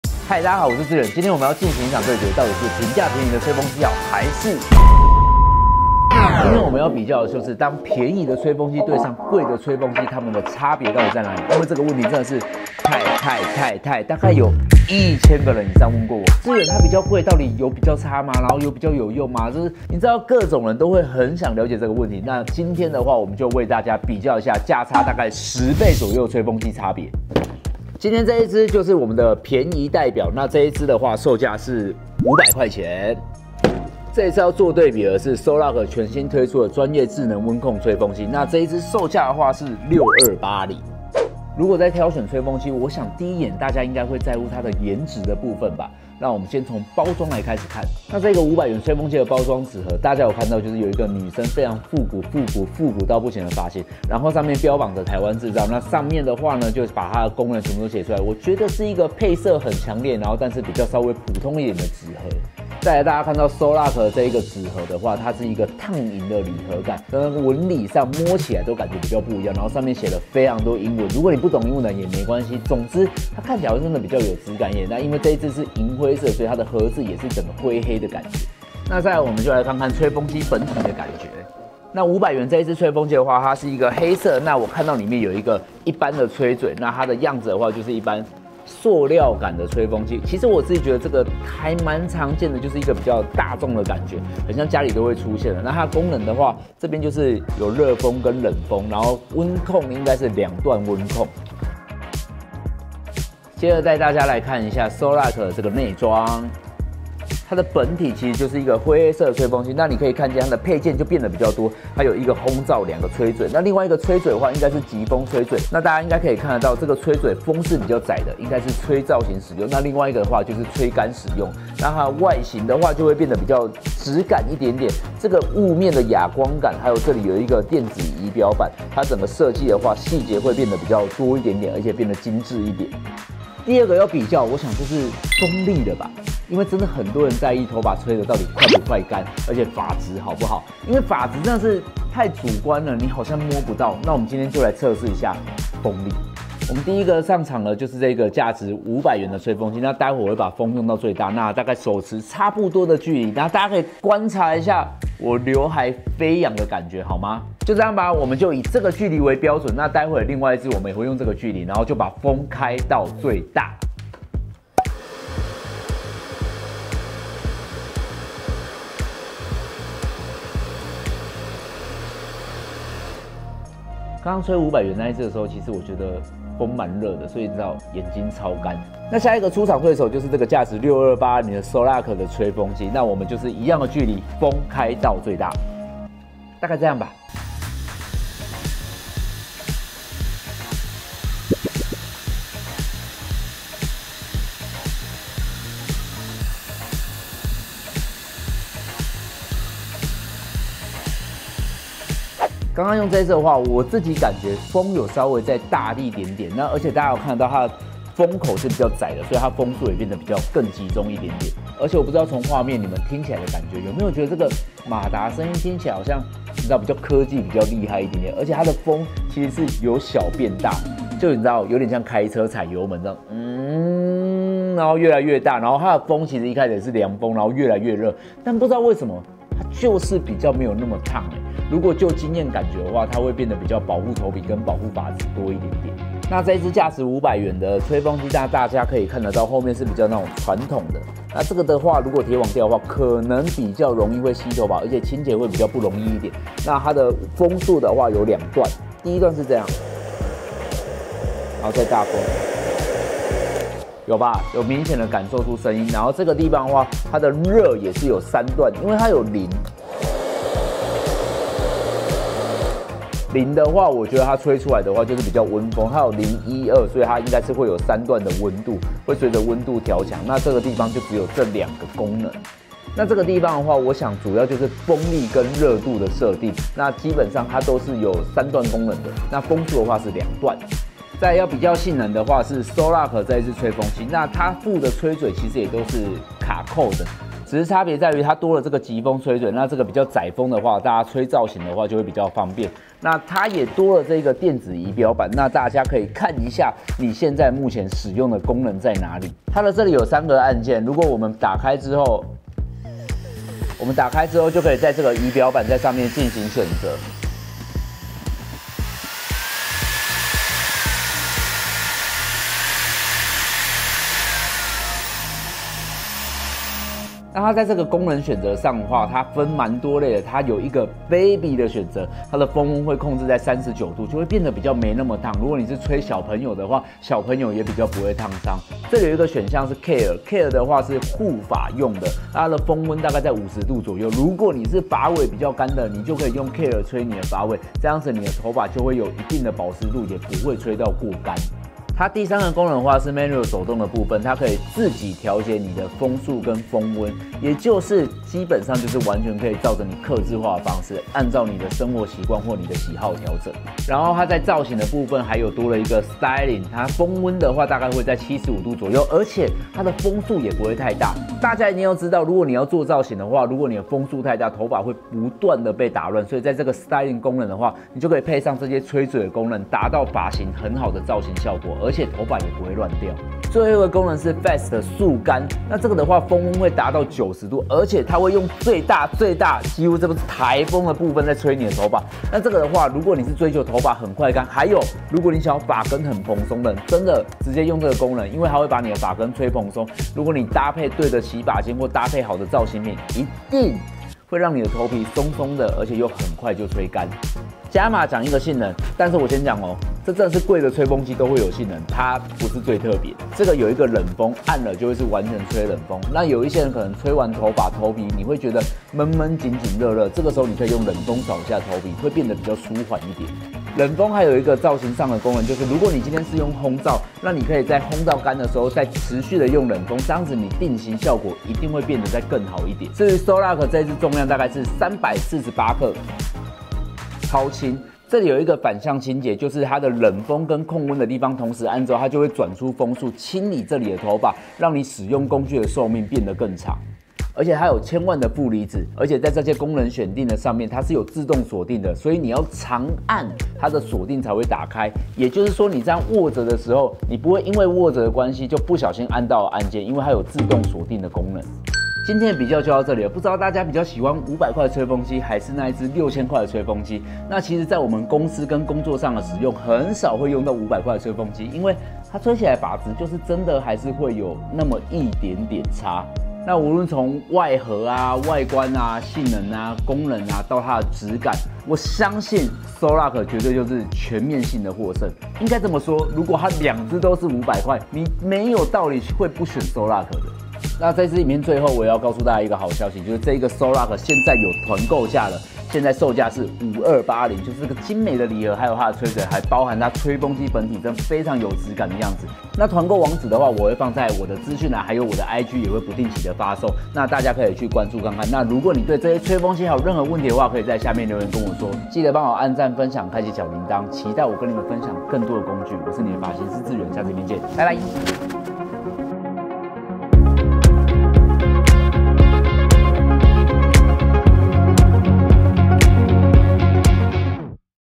評測吹風機吹到發出牛的叫聲？｜你適合哪種吹風機｜10倍價差吹風機對決
sOlac專業智能溫控吹風機
現正團購價只要$5280😍
大家快點擊連結買起來🫶

剪輯：陳立夫 (Lifu_editor)